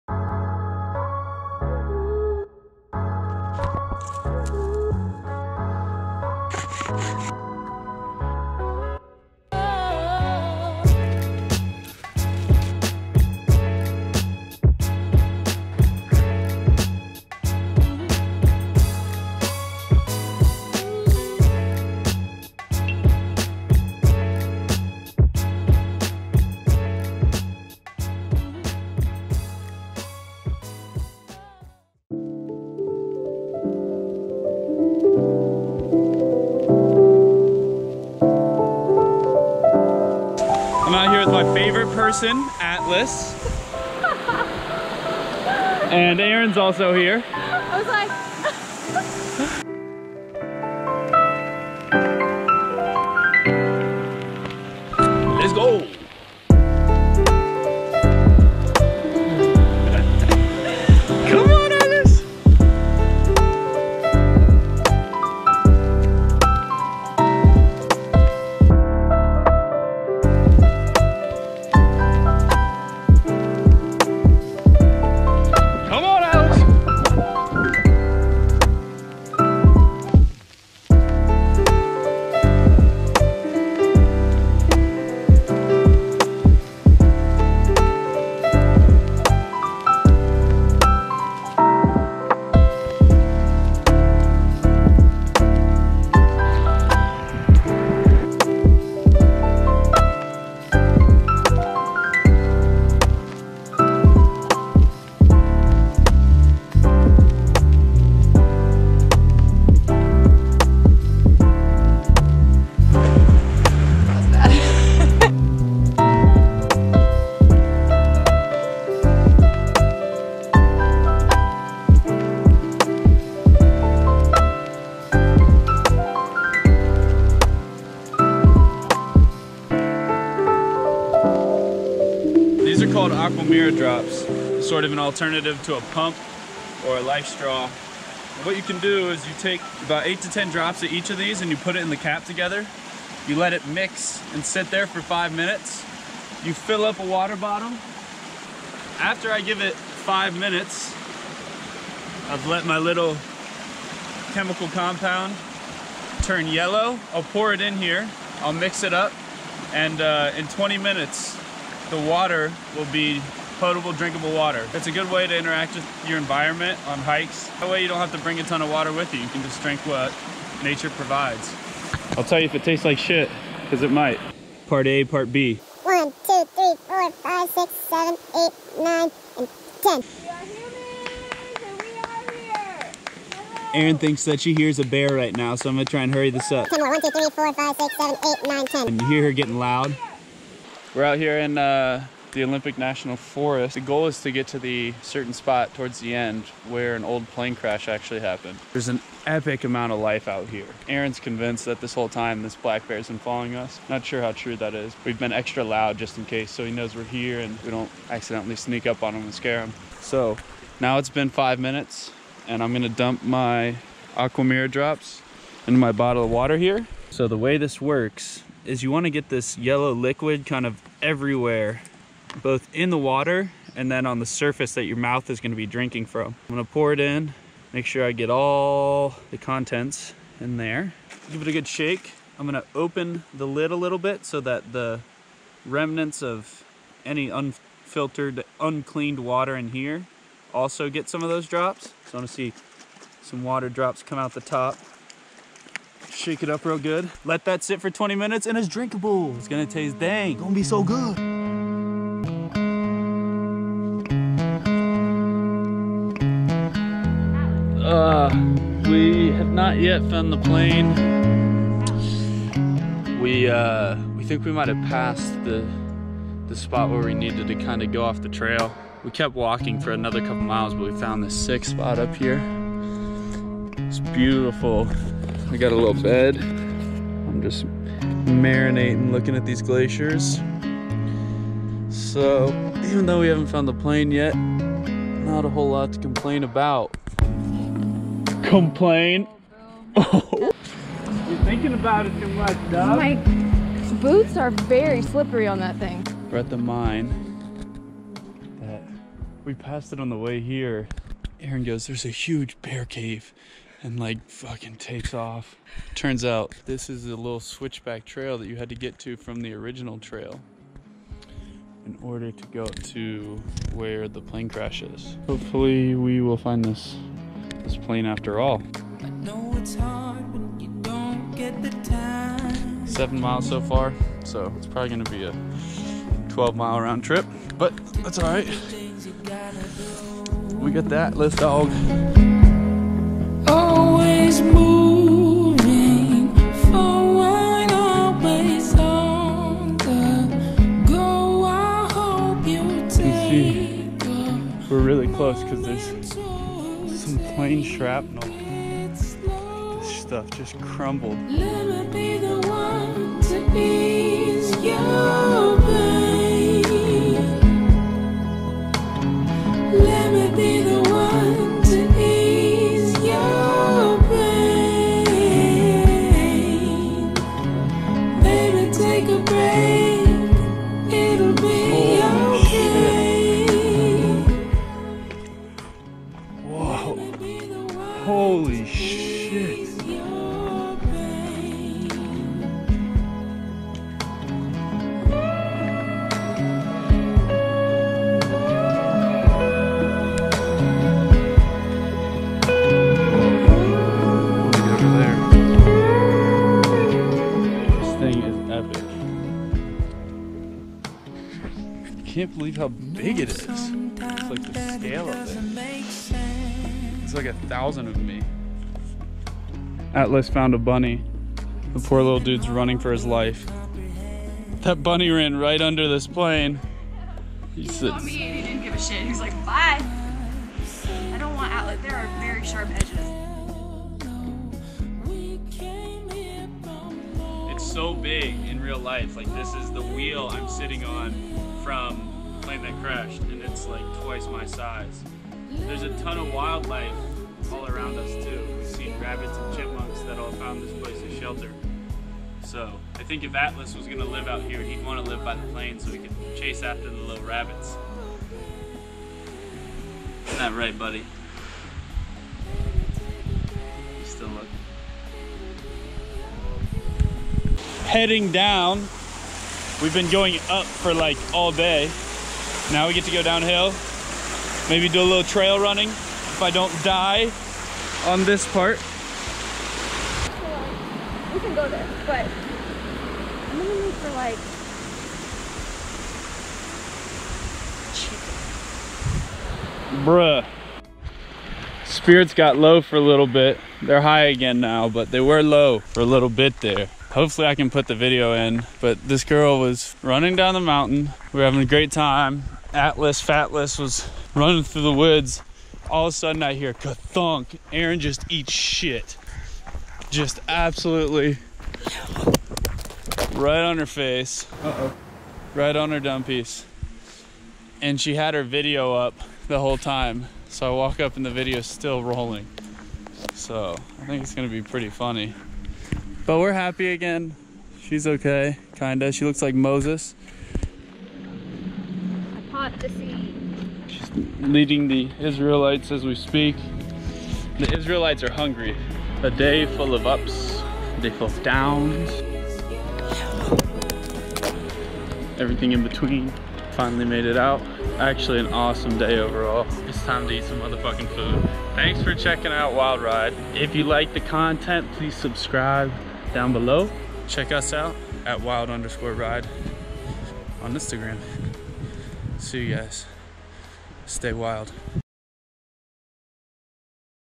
Plane Crash in The Olympic Mountains // WYLD Adventure
In 1952, a B-17 crashed deep in the rugged terrain of Washington's Olympic Mountains. It created a real life survival situation for the 5 survivors of the crash. This is Erin and I's journey to get a closer look.

STAY WYLD